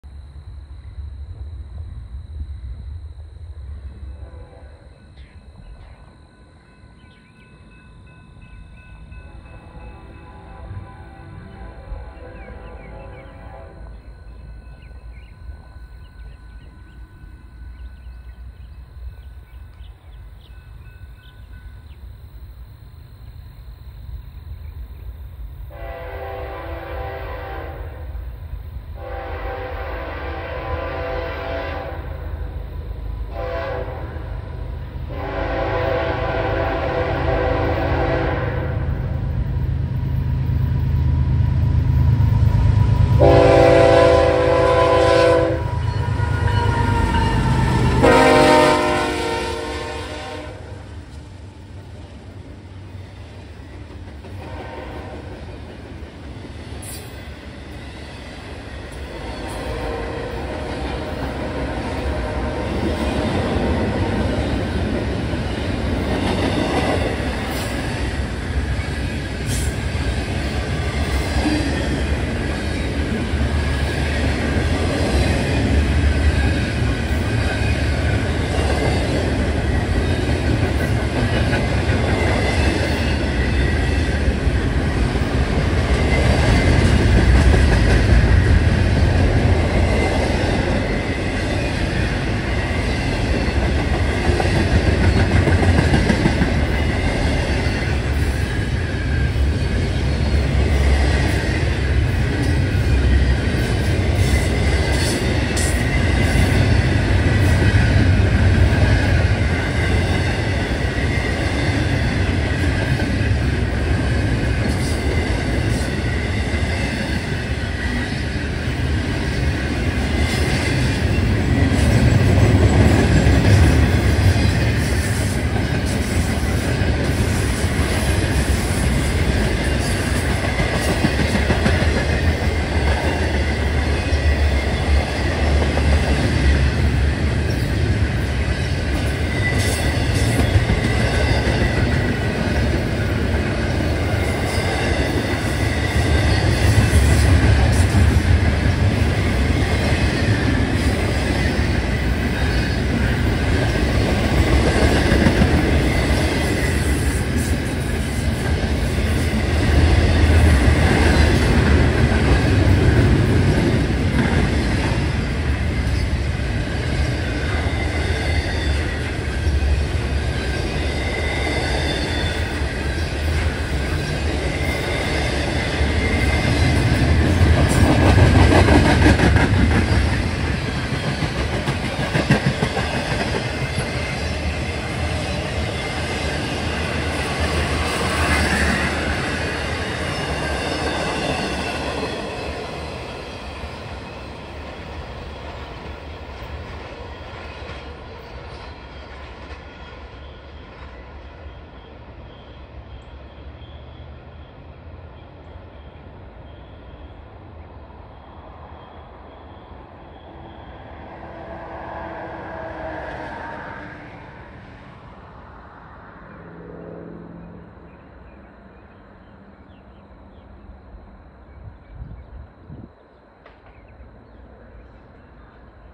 Norfolk Southern 283 (ex-265), 1721 hrs., Tues 4 Oct 2022, Seneca, SC, 77°, 44%, W 5-10, 33300 ft.
Norfolk Southern train 283 (ex-265) at Seneca, South Carolina, 1721 hrs., Tuesday 4 October 2022, 77 degrees F, 44 percent humidity, W wind at 5-10 mph, ceiling unlimited.